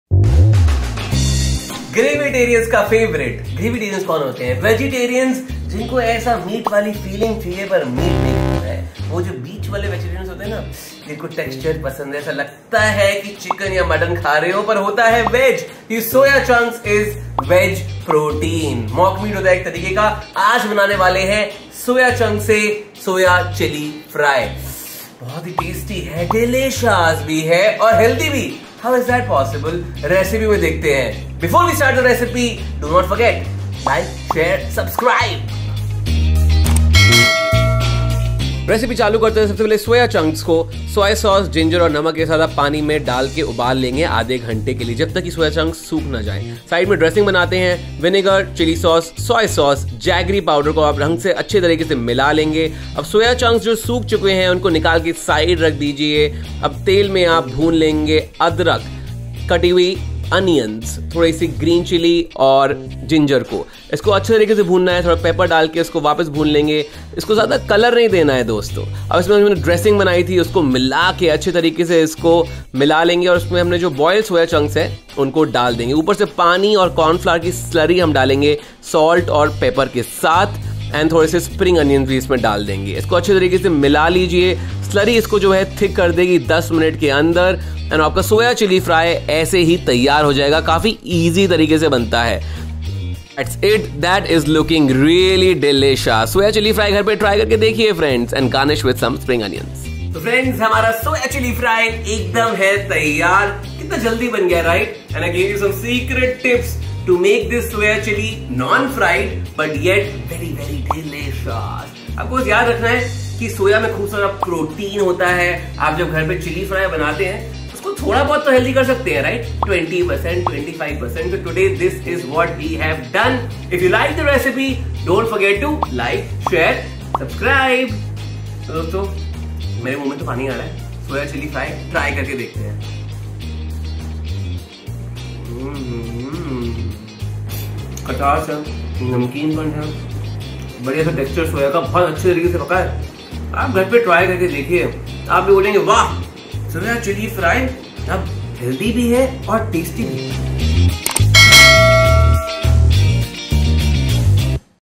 Delicious Protein Rich Soya Chili Fry | हेल्दी सोया चिली ki recipe | @Saransh Goila
Healthy high protein Soya Chilli Fry to make your snacking time nutritious and delishaaas! Whether you are vegetarian, a non-vegetarian or a gravytarian or a vegan, this recipe is going to make anyone of any age drool! Wait no more, note down the recipe.

Recipe:

150g Mini soya chunks
3cups Water
2tsp Light soy sauce
Salt to taste
1inch Ginger
Bring the soya chunks to a boil.

Dressing for Soya Chili

1/4cup Rice wine vinegar
2tbsp Chili sauce
2tbsp Light soy sauce
1tbsp Jaggery powder
Mix well

All water should be completely soaked by the soya chunks.

- Heat a pan and add
- 2tsp Sesame oil
- 2tsp Garlic, chopped
- 1 Onion, diced
- 2 Green chilies, sliced
- 1/2 inch Ginger, chopped
- 1 Green capsicum, diced

Add the sauce & mix well.
Add the Soya chunks and toss on high flame.
Add 1/4cup water, mix well.
Add crushed black pepper to taste, a handful of spring onion greens & 2tsp of chopped coriander mix well.
Add 1 tbsp corn flour slurry, mix well. Adjust the seasoning by adding salt to taste.
Serve hot

Nutrition value per serve
Energy - 217 kcals
Carbs - 20.5 gms
Protein - 7.5 gms
Fat - 12.5 gms